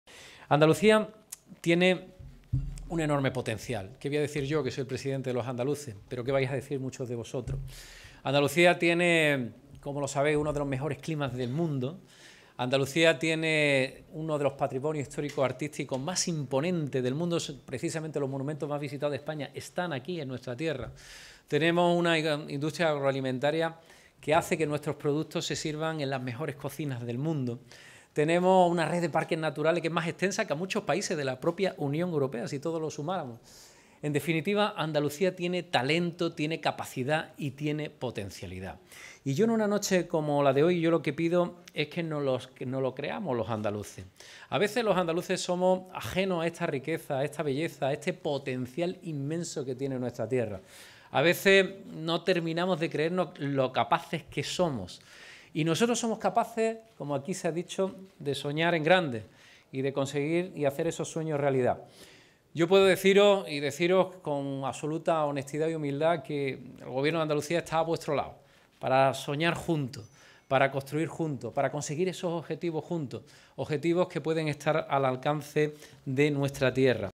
Moreno dice que Andalucía tiene que "creerse" todo su potencial
El presidente de la Junta de Andalucía, Juanma Moreno, ha apuntado que Andalucía es una gran potencia a la que solo le falta "creérselo" y desplegar todo su potencial. De esta manera, ha asegurado que el Gobierno andaluz está empeñado en poner en marcha Andalucía, en "reactivarla, despertarla del letargo y el hastío detectado en tantos sitios".